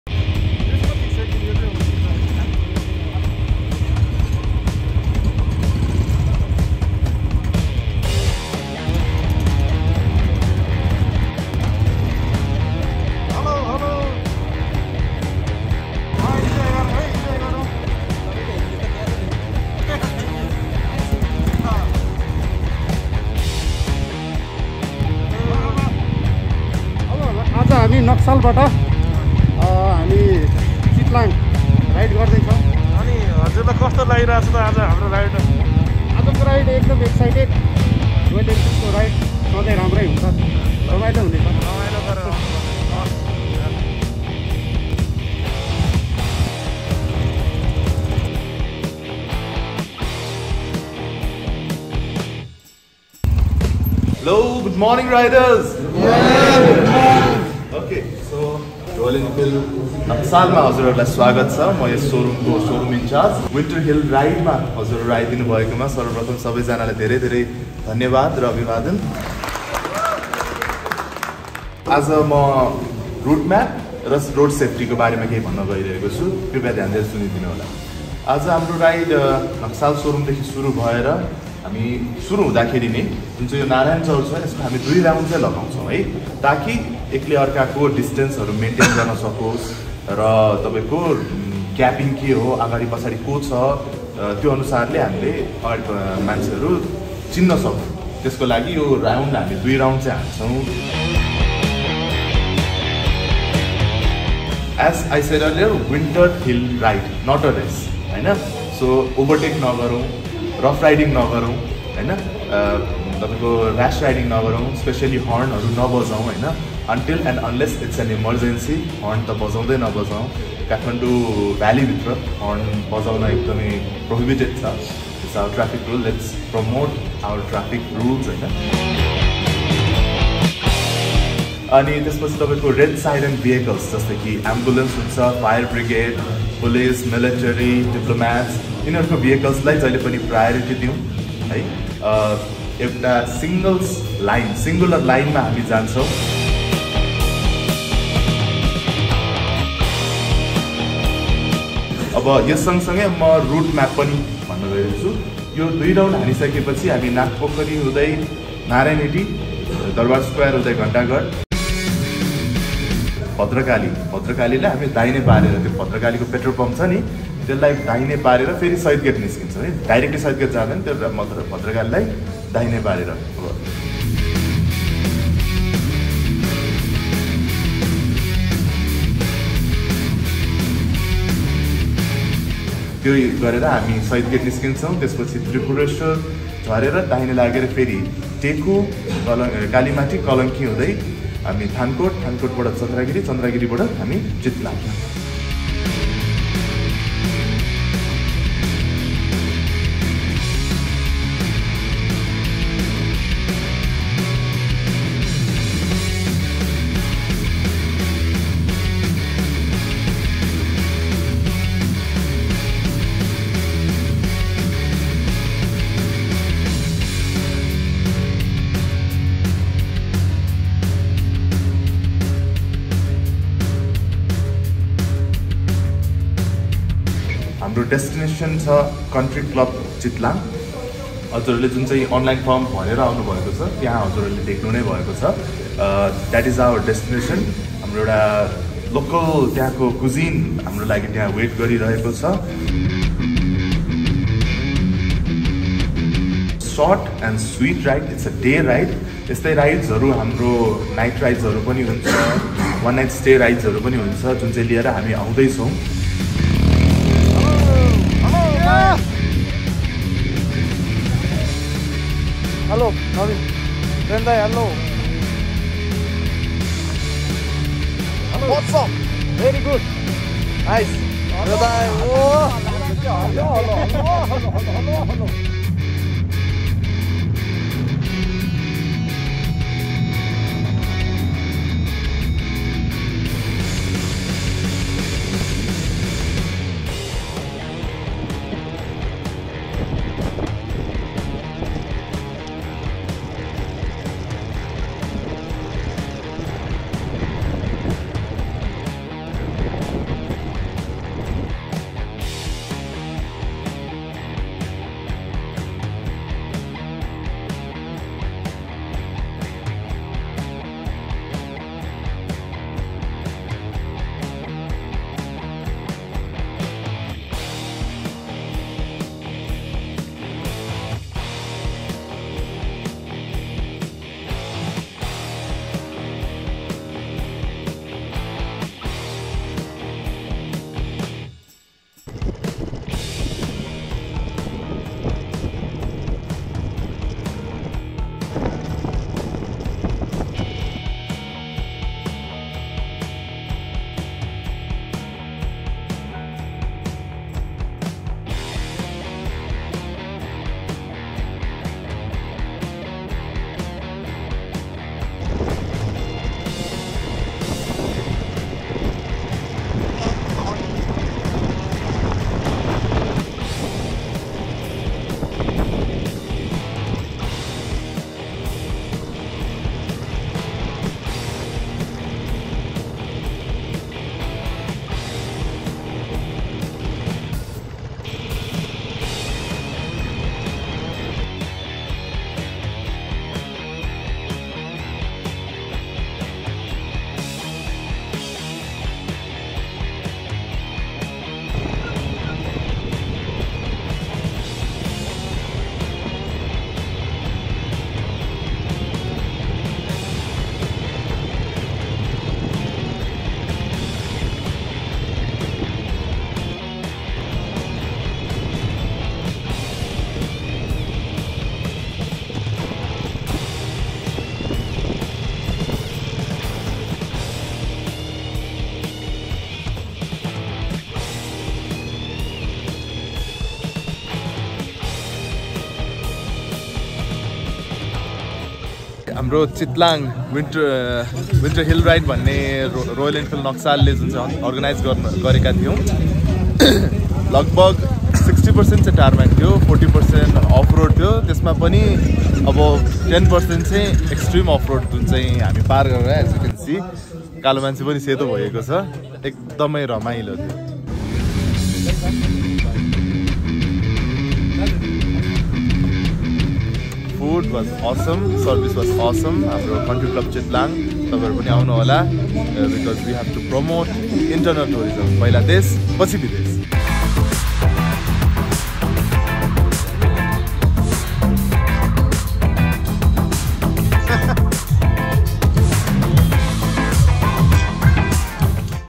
Royal Enfield Winter Adventure: Journey From Kathmandu To Chitlang l चित्लाङ्ग राइड
Royal Enfield Winter Hill Ride, Royal Enfield Showroom Naxal Kathmandu to Chitlang.
23rd Dec 2023
काठमाडौ, रोयल एन्फिल्डको आधिकारिक बितरक अल्फा अटोमोटिभले चित्लाङ्ग राइड ( Winter Hill Ride) गराएको छ।
सनिबार बिहान ९ बजे सुरु भएको राइडमा ५४ रोएल एन्फिल्ड मोटरसाइकलमा ६५ जनाको सहभागिता रहेकोे थियो ।
अल्फा अटोमोटिभको सोरुम नक्सालबाट सुरु भएका राइड शोरुमदेखि दरबार मार्ग, शहीदगेट, त्रिपुरेश्वर, कालीमाटी, सोल्टिमोड, कंलकी चन्द्रागिरि हुँदै चित्लाङ्ग पुगेको थियो ।
आन्तरिक पर्यटन प्रवद्र्धनका साथै रोड सेफ्टी अवेर्नेस, प्रमोटिङ सिस्टमेटिक राइड गर्ने उद्देश्यले उक्त कार्यक्रमको आयोजना गरिएको कम्पनीले जनाएको छ ।